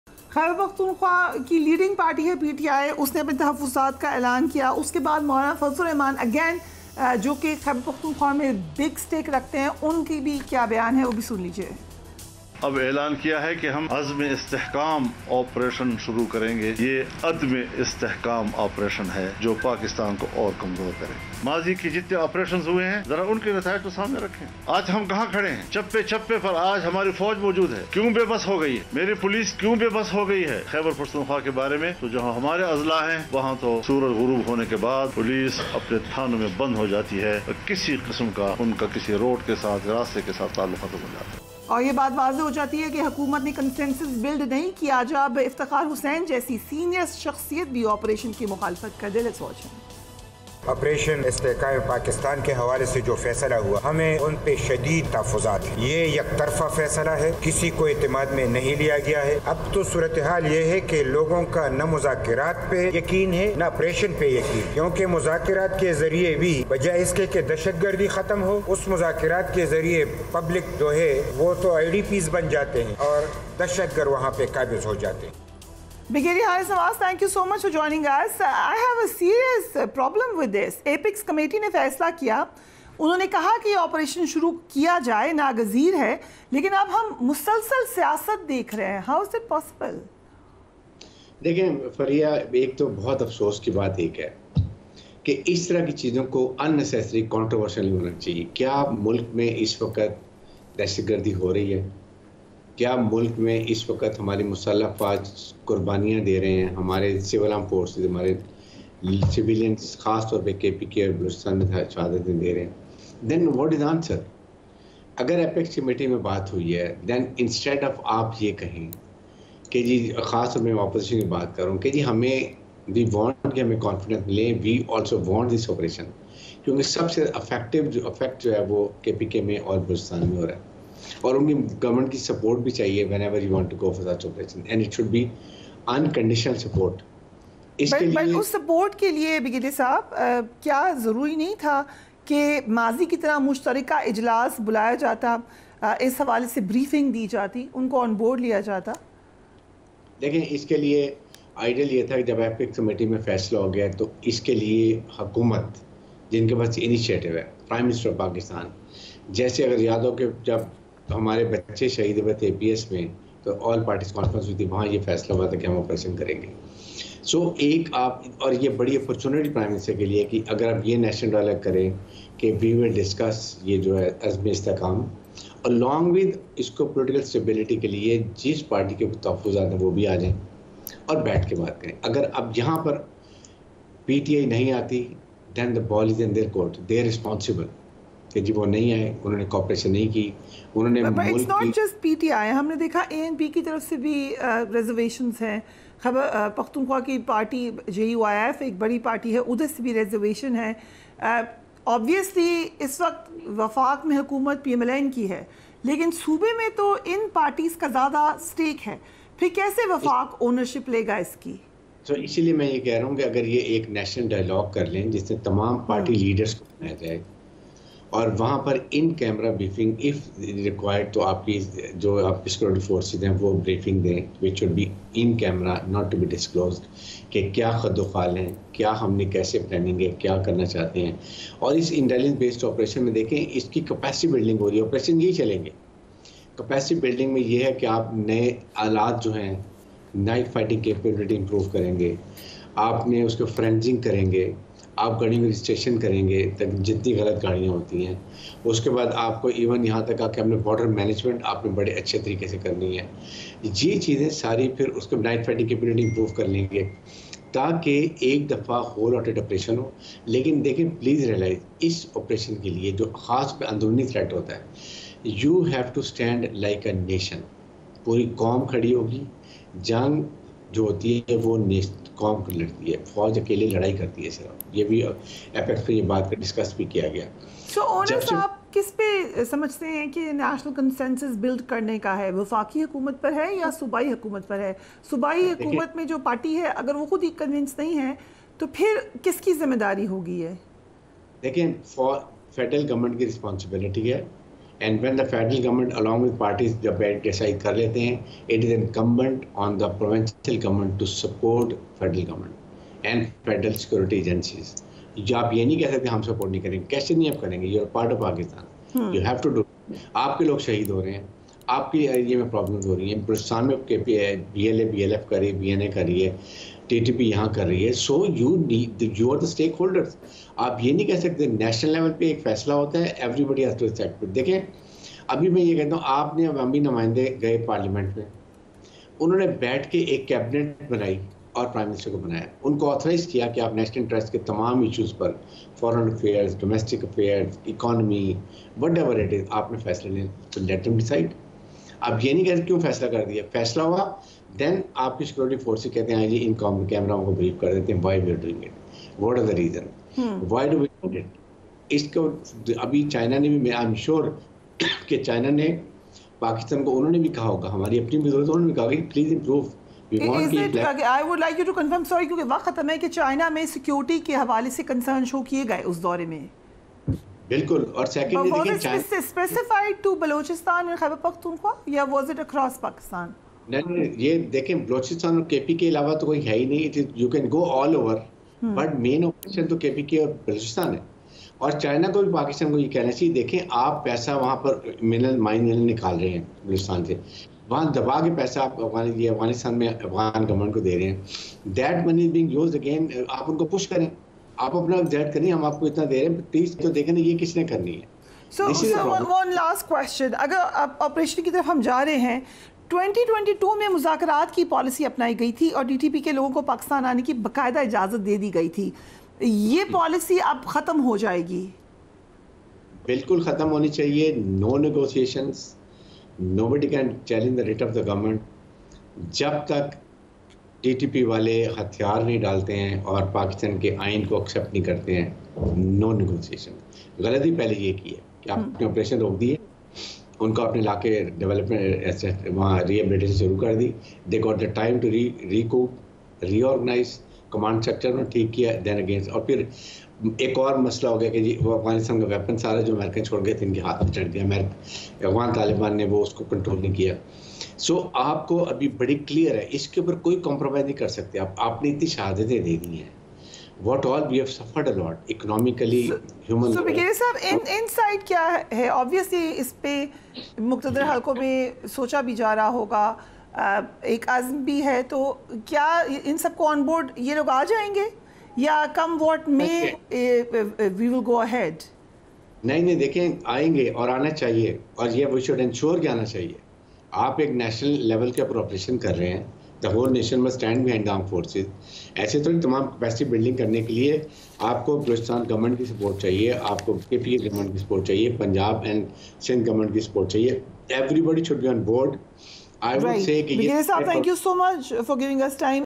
What is The Real Truth of Operation Azm-e-Istehkam? | Brig (r) Haris Nawaz Opened New Pandora Box
What is The Real Truth of Operation Azm-e-Istehkam? | Brig (r) Haris Nawaz Opened New Pandora Box | GNN